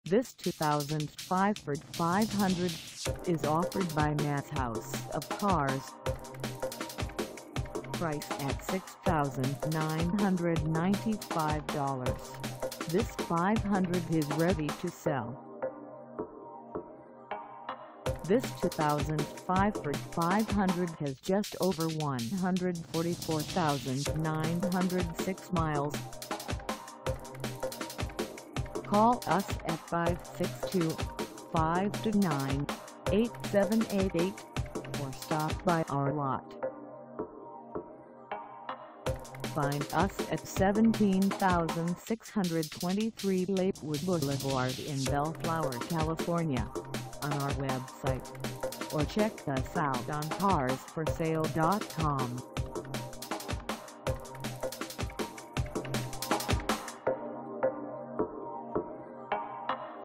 2005 Ford Five Hundred Limited AWD 4dr Sedan for sale in Bel
This 2005 Ford Five Hundred Limited AWD 4dr Sedan is for sale in Bellflower, CA 90706 at Maz House of Cars.
Engine: V6 3.0L V6
Transmission: CVT
Color: BLACK
Mileage: 144906
Contact Maz House of Cars 17623 Lakewood Blvd Bellflower, CA 90706 to test drive this 2005 Ford Five Hundred Limited AWD 4dr Sedan today.